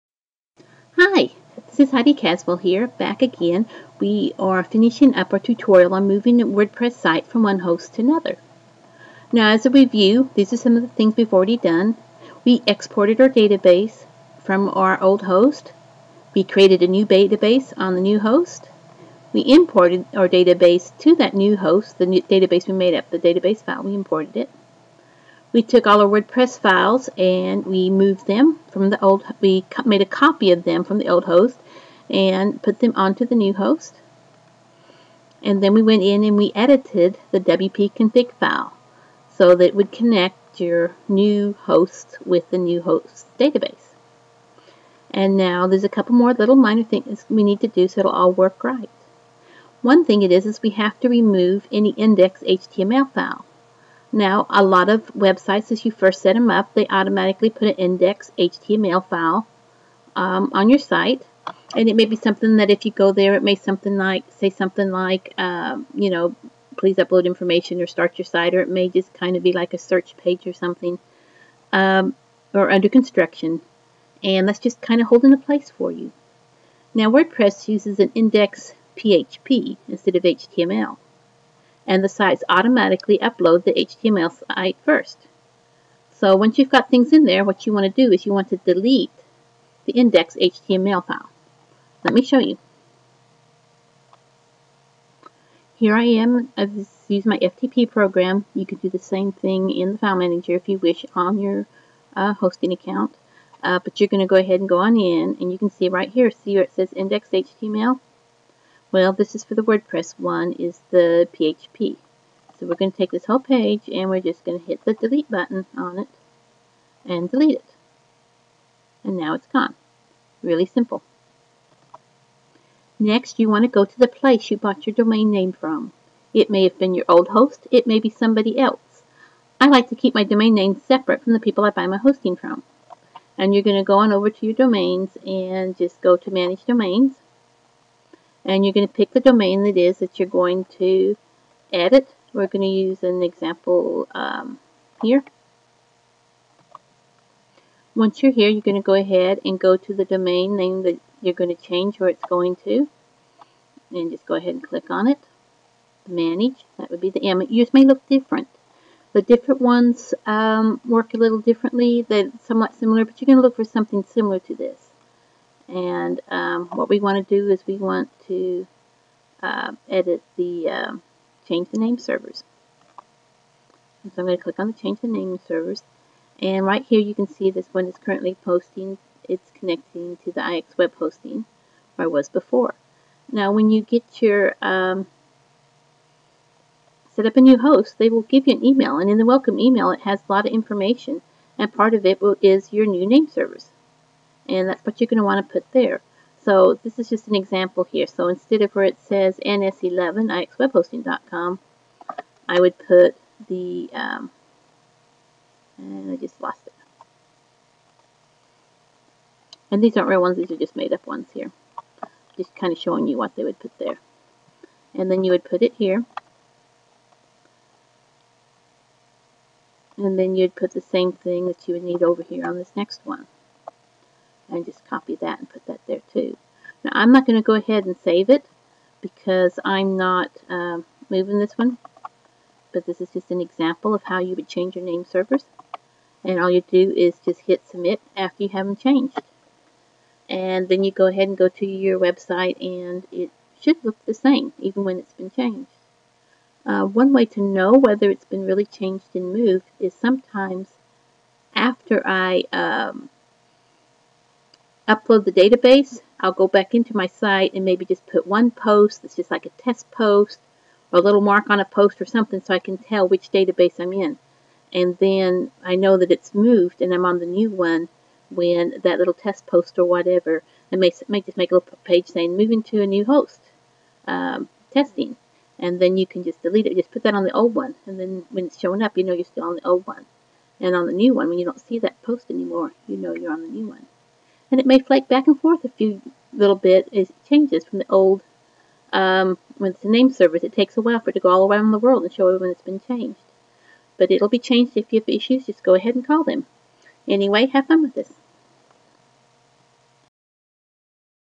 Delete index.html and change name servers
To move Wordpress to a new host, I like to copy both the files and the database to the new host.  This video show how to delete any extra index.html pages and change the name servers.